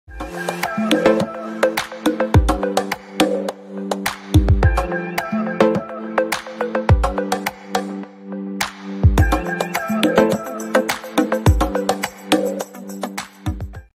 Stem Street at Spraoi.MOV
Maths Ireland takeover Hanover St in Waterford for Spróg, bringing all the Fun of Maths into the heart of the city - two days of fantastic family entertainment and wonder